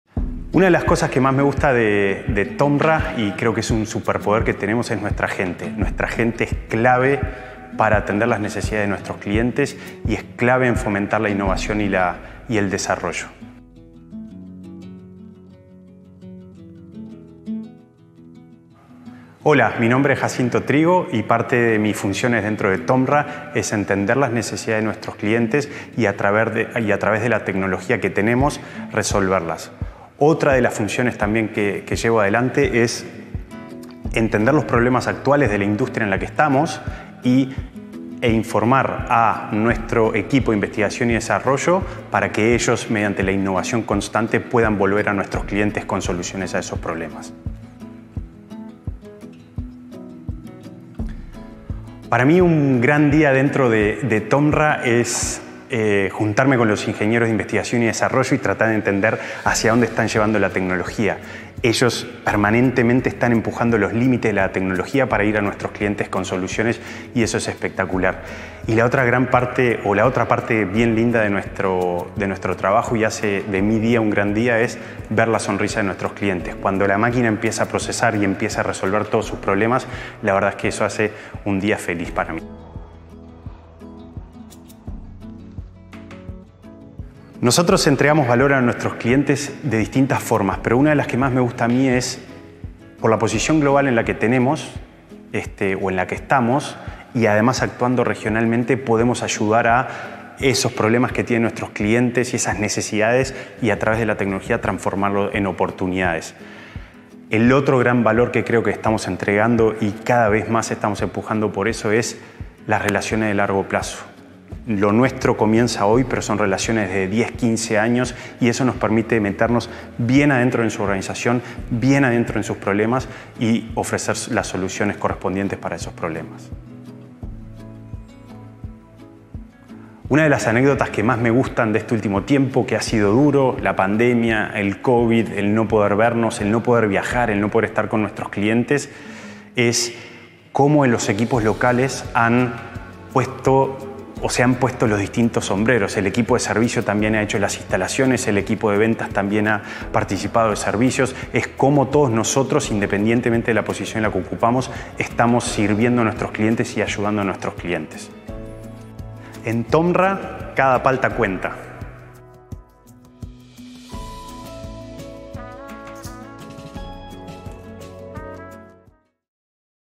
Employee Boost Movie - Jacinto
It’s no secret that we have amazing talent at TOMRA. But did you know some of us are also talented in front of a camera? We recently launched our Employee Boost movies where colleagues talk about TOMRA Food, their job, and what drives them every day.

Each week we'll release an Employee Boost Movie. This week, meet Jacinto Trigo, Global Category Director | Avocados